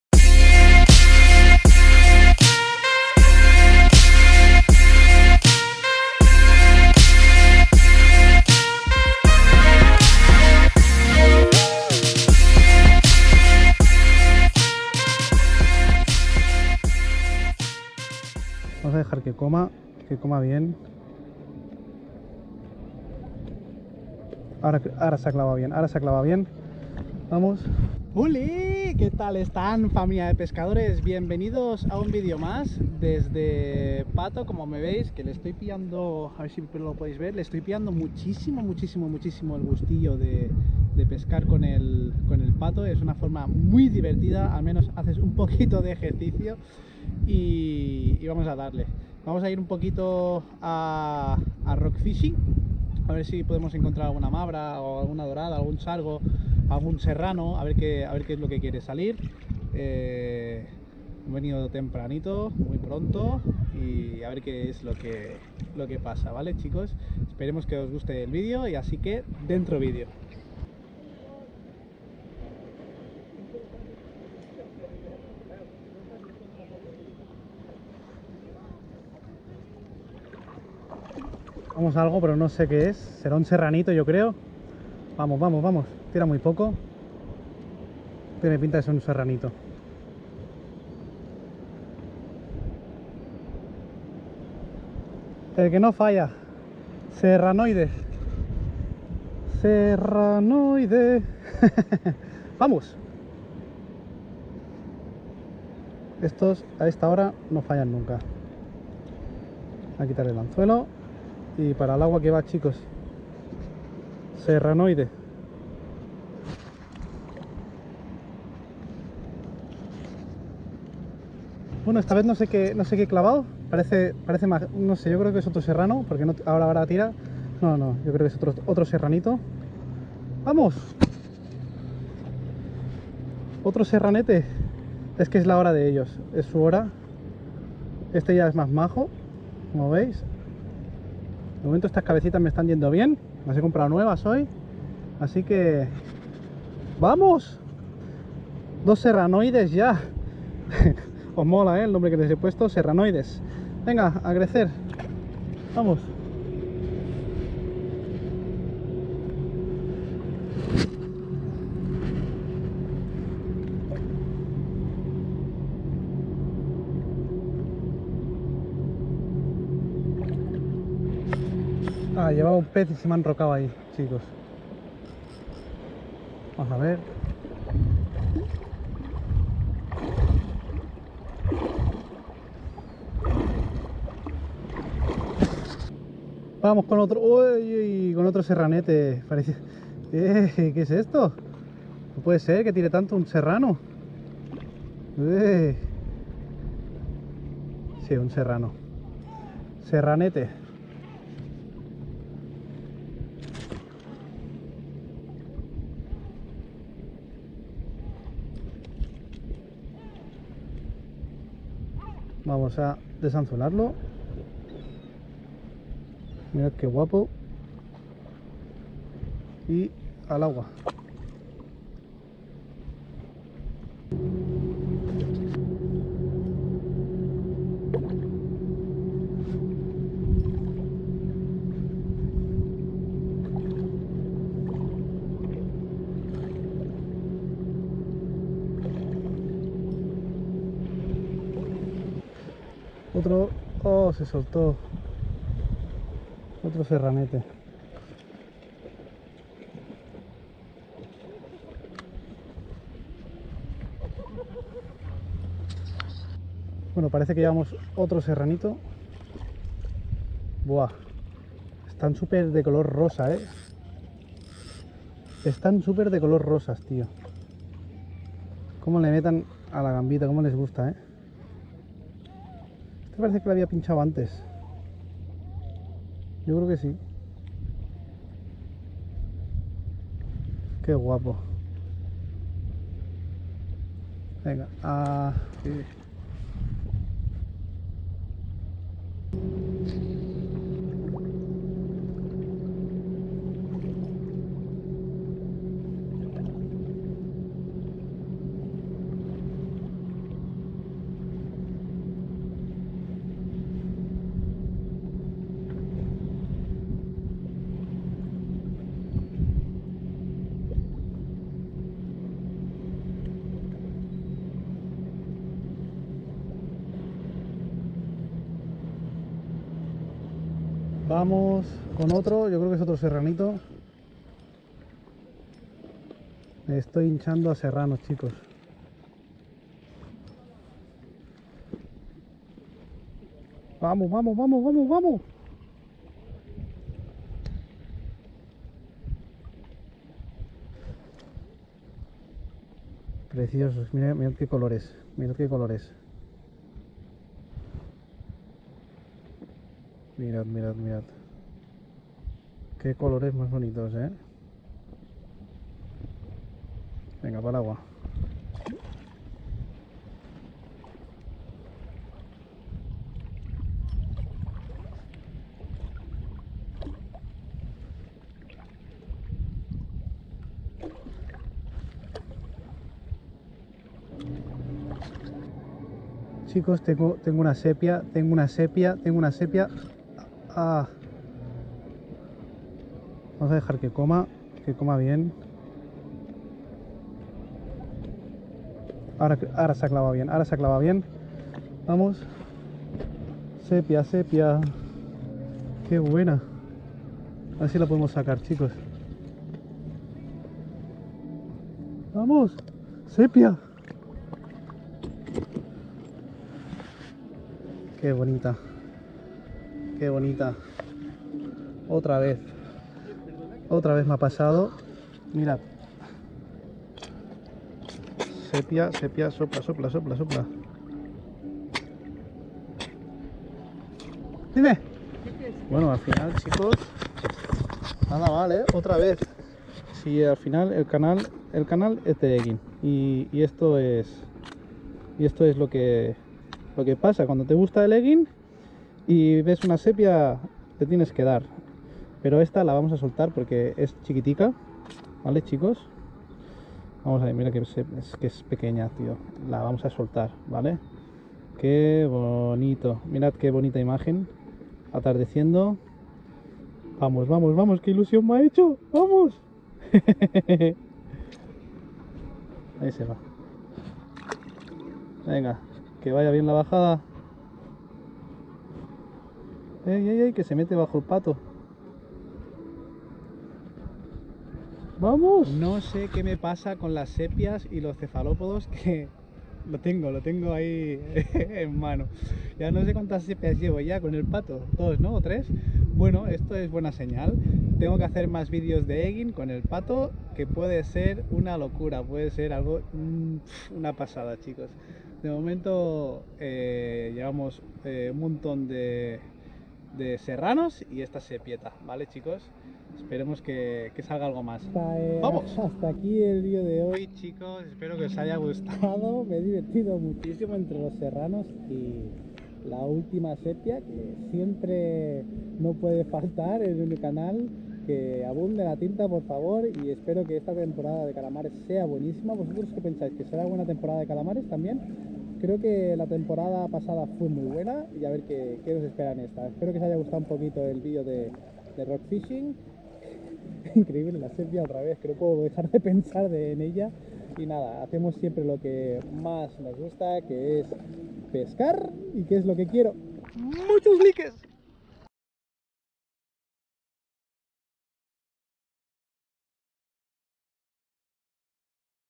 SEPIAS desde PATO y con VINILO de ROCKFISHING de LOCOS!!
Sepias desde pato y con vinilo de rockfishing de locos un día mas para recordar que las sepias les encanta ese vinilo.
Deciros que las pesca desde pato me esta encantando ya que muy divertido el poder estar flotando y a la misma vez flotando.

Comentaros que vienen más videos muy guapos y que no os lo perdáis.

 𝟏𝟎 % 𝑫𝑪𝑻 𝑪𝑶𝑵 𝑬𝑳 𝑪Ó𝑫𝑰𝑮𝑶: 𝑭𝑹𝑨𝑵𝑪𝑭𝑰𝑺𝑯𝑰𝑵𝑮𝟏𝟎